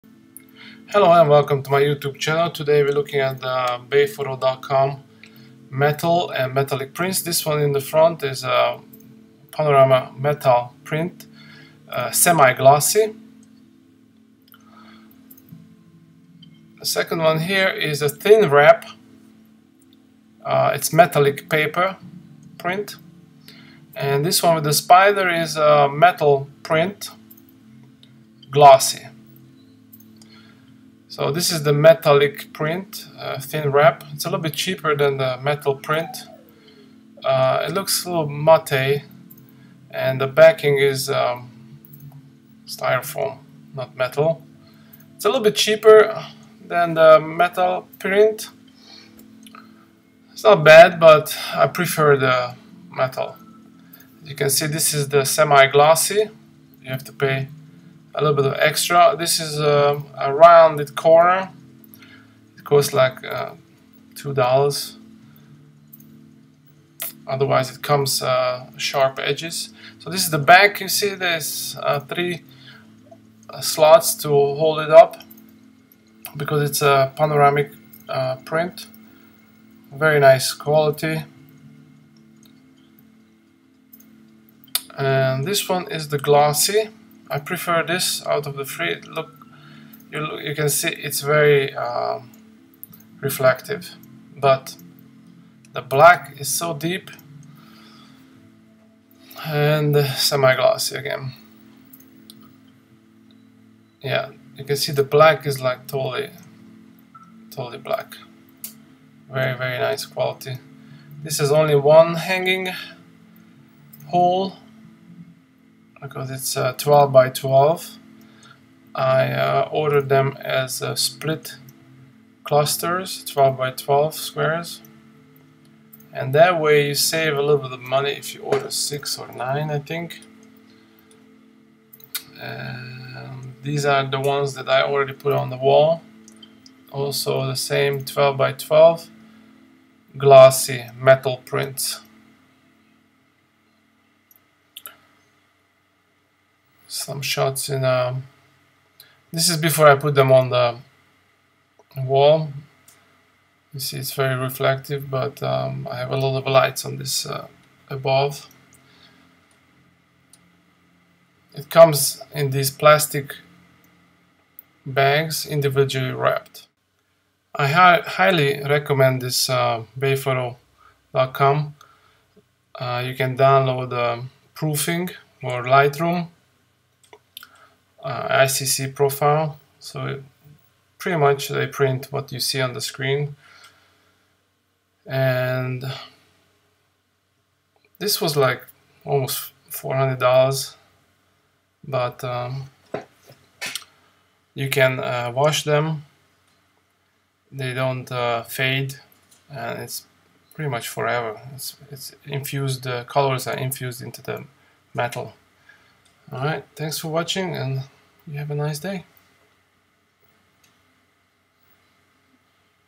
Bayphoto.com Metal Prints and Thinwrap metallic print 4K
12x12 Metal prints and Thinwrap metal paper print.
Clusters and splits are supposed to be 20% cheaper than individual prints,but i can't confirm that.
Amazing quality metal prints!